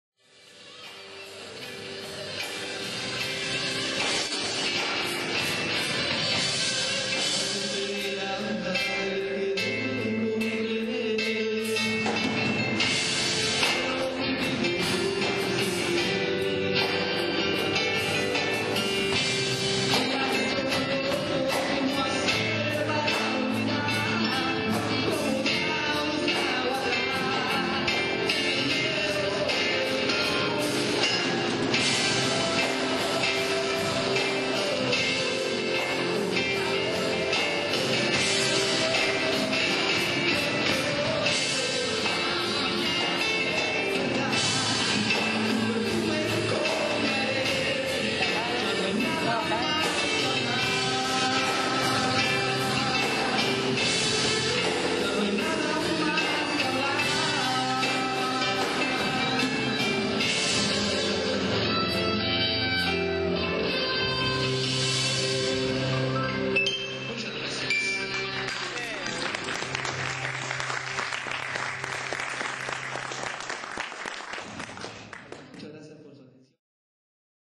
faena en cantatutores peru 2006
jose luis espinoza y su banda faena en cantautores 2006 en trujillo peru